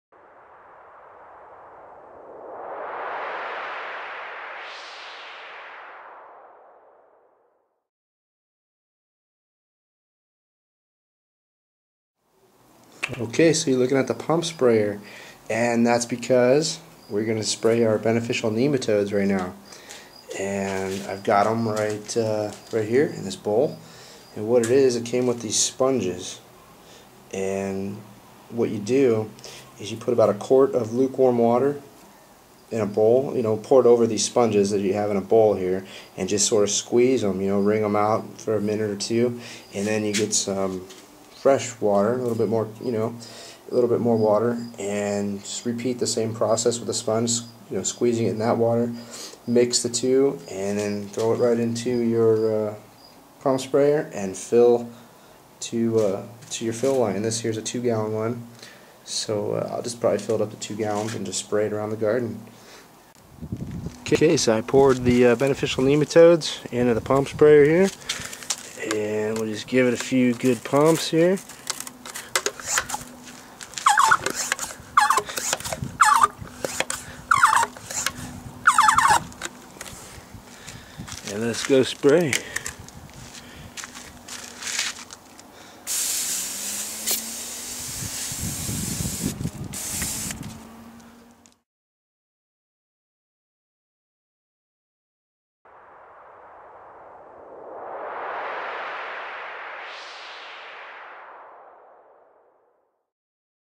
Spraying beneficial nematodes in the garden
A real fast video talking about and showing how to spray beneficial nematodes onto your garden soil. I am hoping that these will help me in my battle agaisnt the grubs I've been finding.